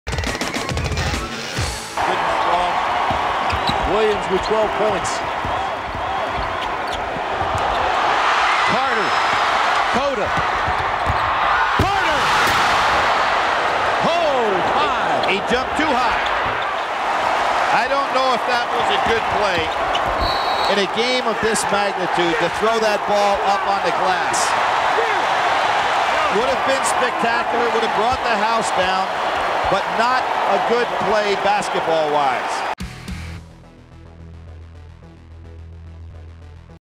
Vince Carter's Best Missed Dunk Of All Time | ACC Hidden Gem
Vince Carter dunks have been known to bring down the house, but one that never happened just might be among his best.  In his final season at North Carolina, the Tar Heels were well ahead of rival Duke late in the 2nd half when Ed Cota lobbed a pass off the backboard on the fast break.  The end result was almost one of the best dunks of Vinsanity's career at Carolina.